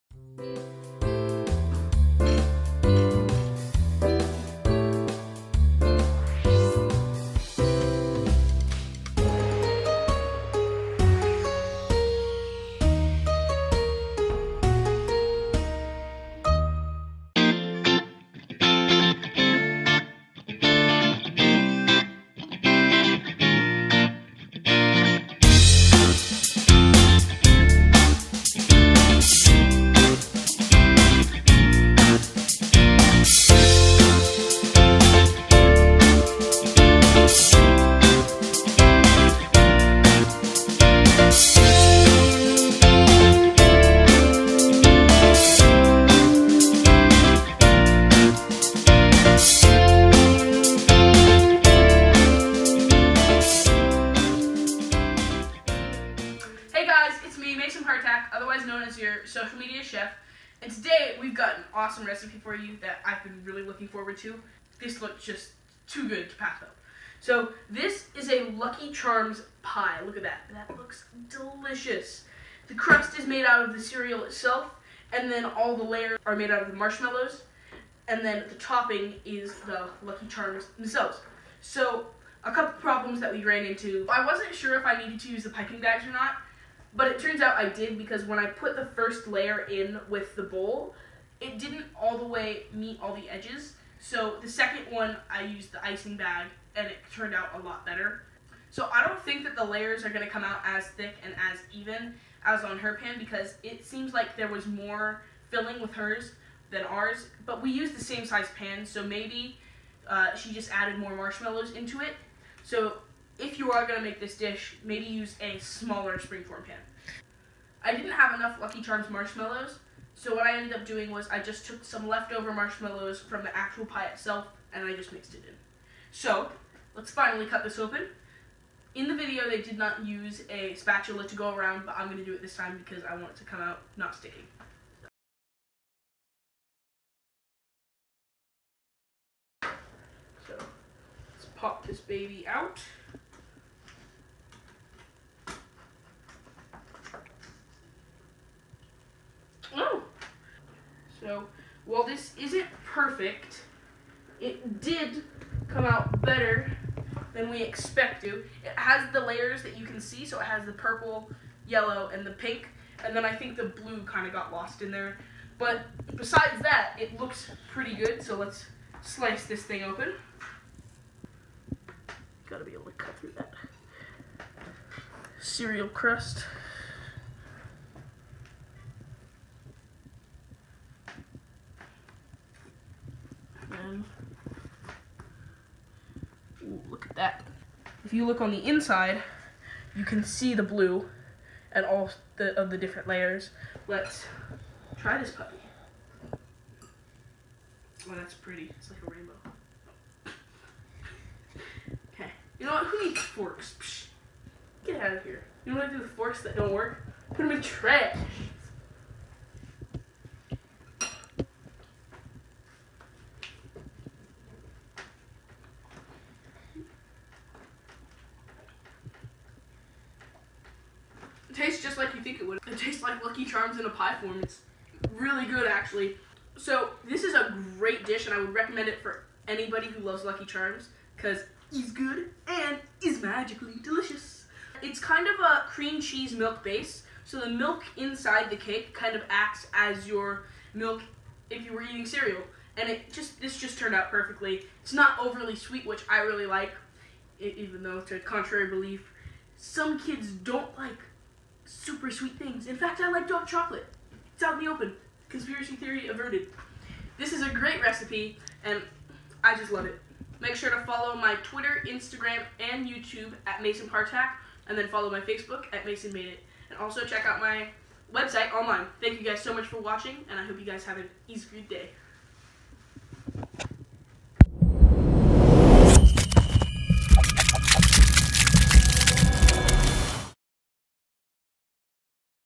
Lucky Charms Pie
I couldn't wait to try this recipe, check it out and see what you think. Let me know what recipes you want to see me review on my channel and I'll put them on my list.